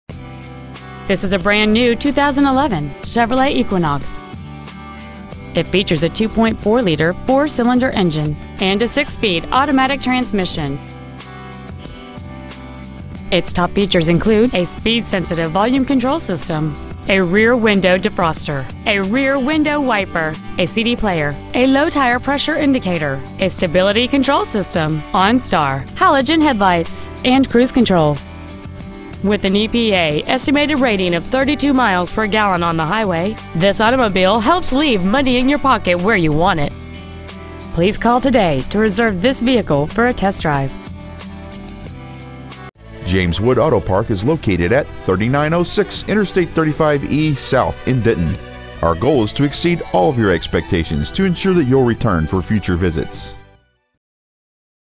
2011 Chevrolet Equinox Decatur TX
Year : 2011
 Make : Chevrolet
 Model : Equinox
 Engine : 2.4L 4 Cyl. Sequential Fuel Injection
 Trans . : 6 Speed Automatic
 Exterior : Summit White
 Miles : 10

 Stock : 213246

 2011 Chevrolet Equinox Denton TX